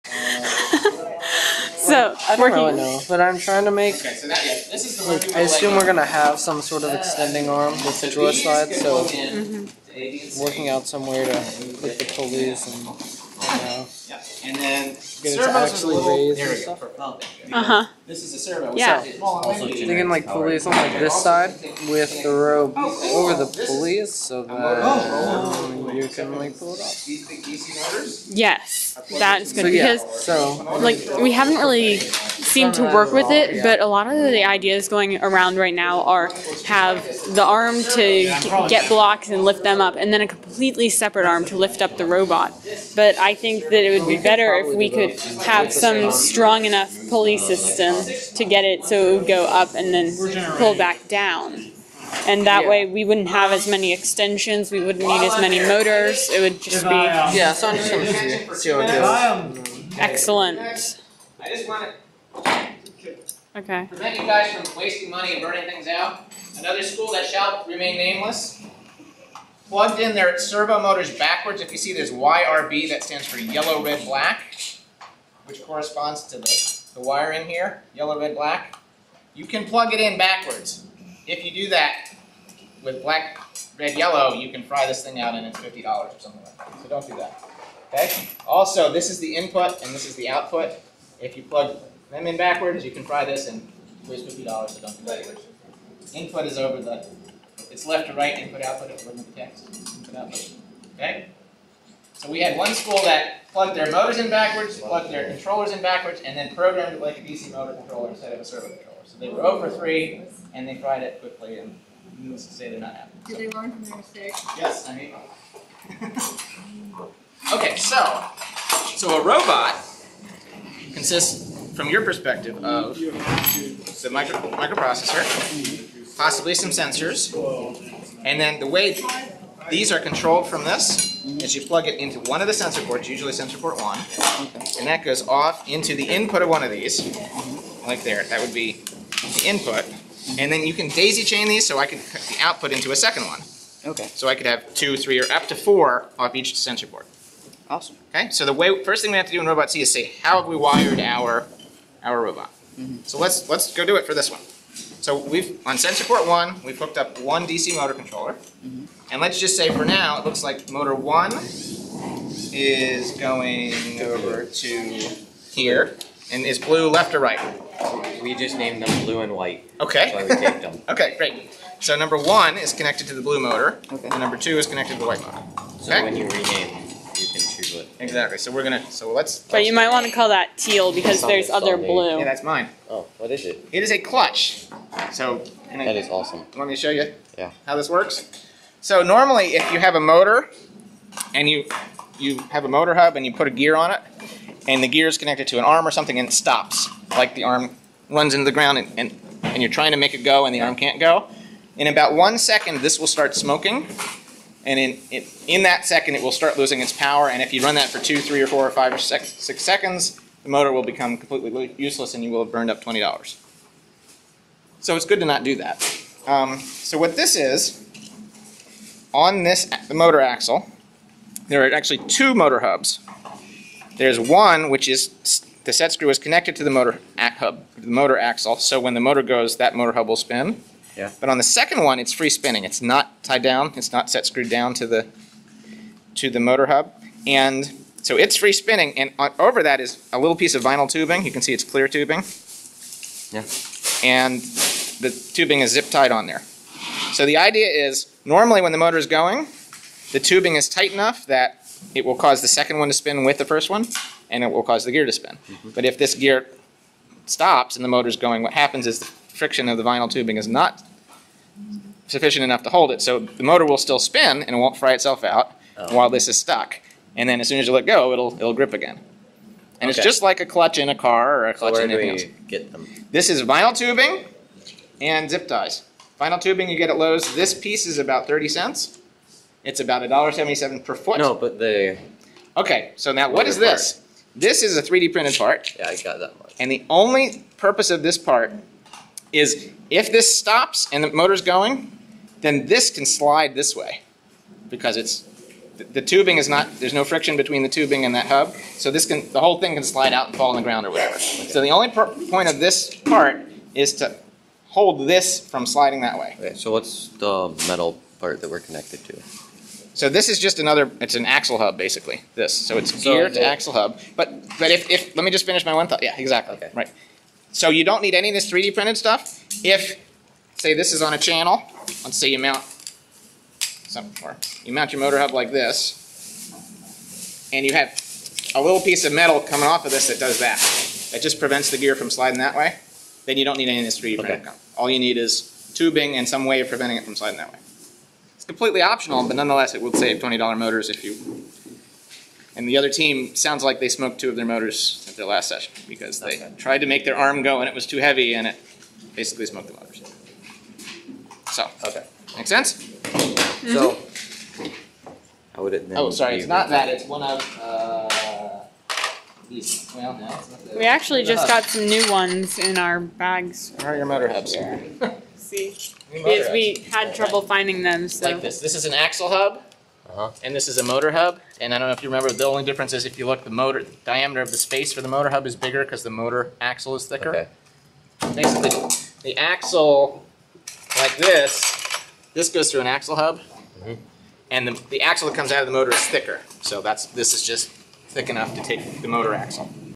Nov. 13, 2013: A pulley for the drawer slides
An alternate to using a rack gear on the drawer slides is a pulley system, so Emmet is working on a system to both extend and retract the arm.